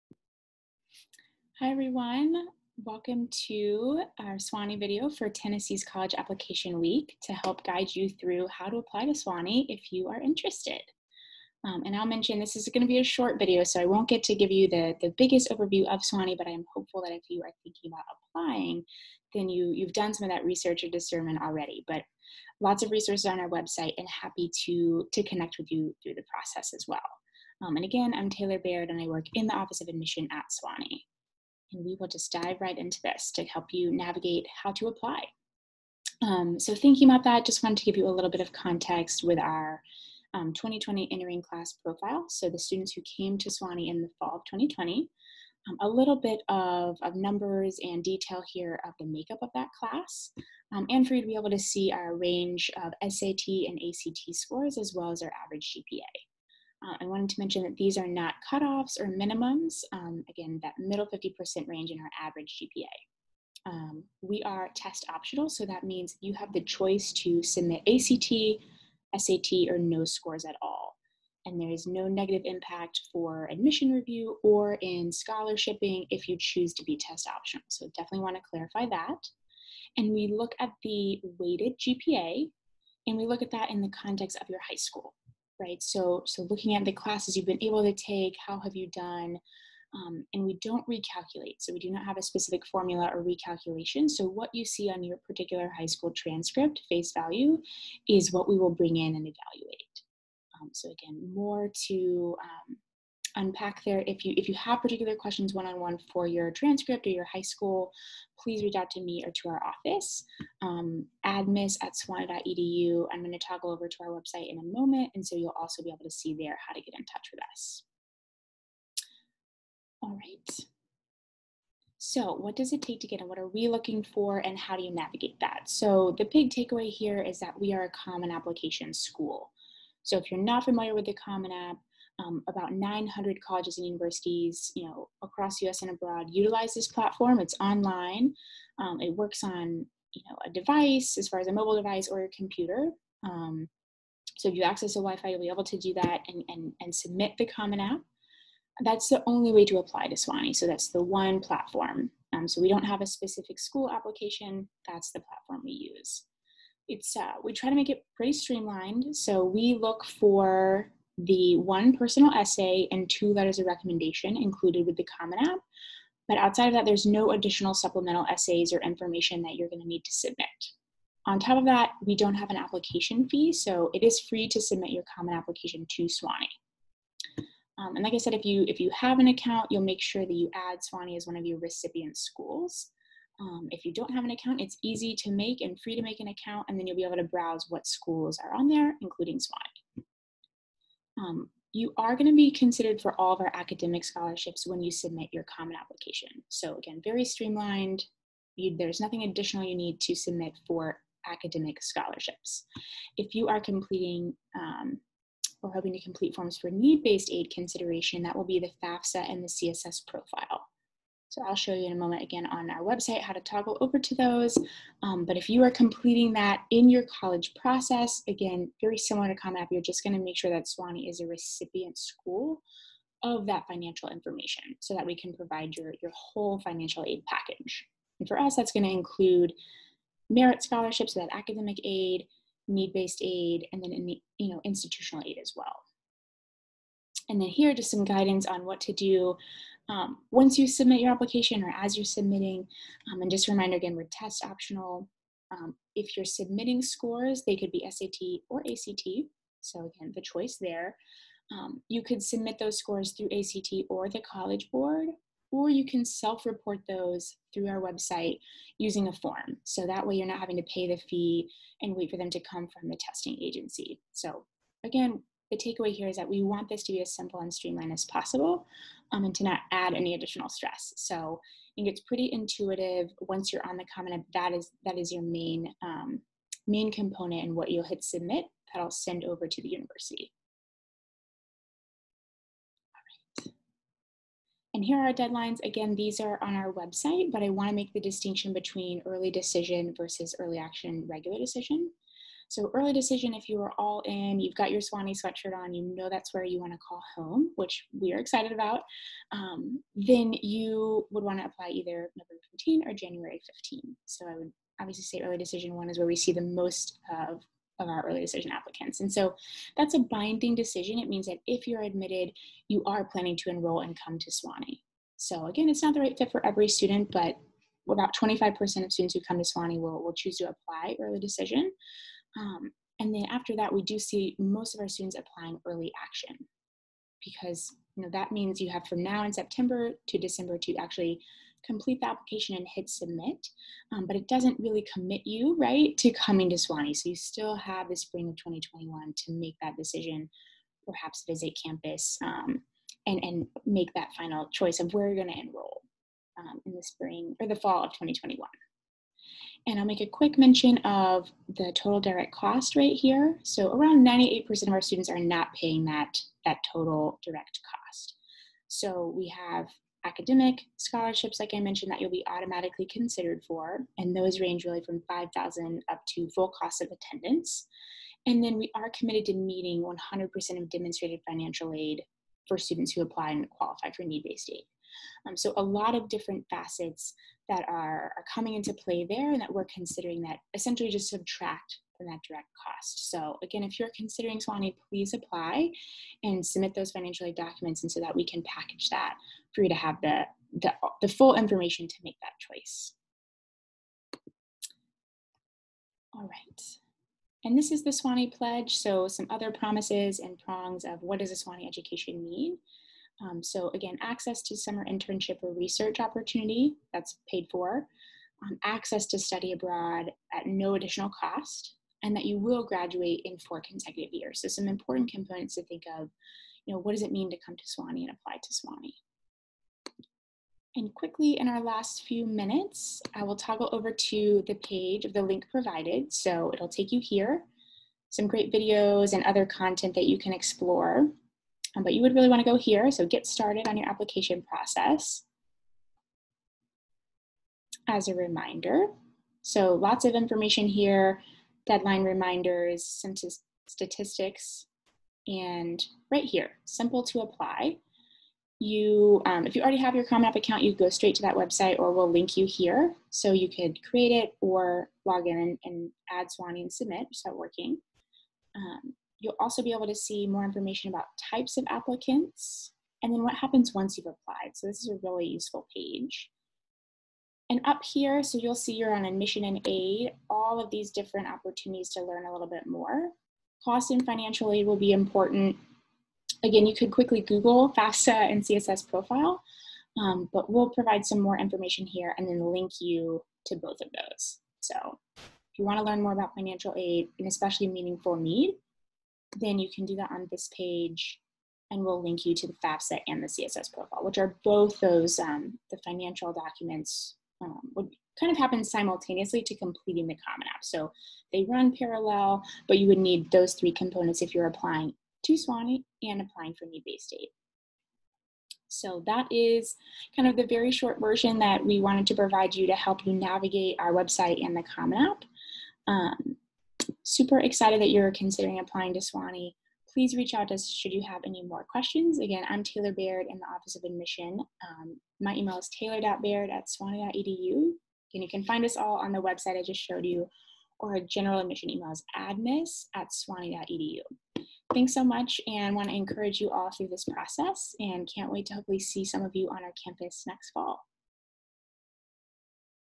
Sewanee -The University of the South Application Video (21/22)
This video will walk you through how to apply to Sewanee -The University of the South!